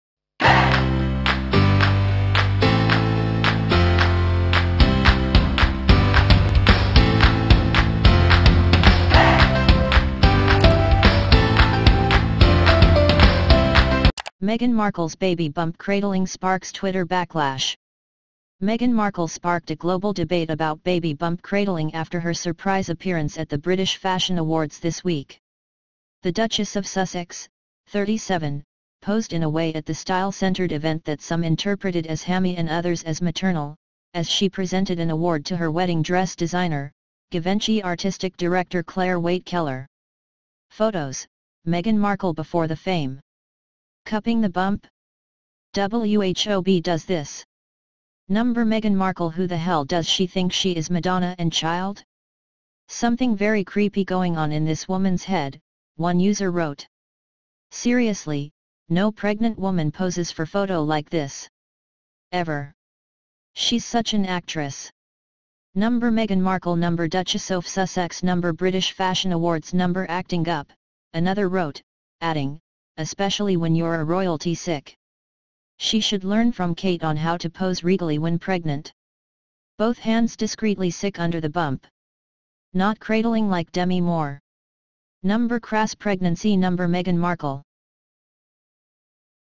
FOXNEWS: Meghan Markle's baby bump cradling sparks Twitter backlash BREAKING NEWS
BREAKING NEWS Meghan Markle's baby bump cradling sparks Twitter backlash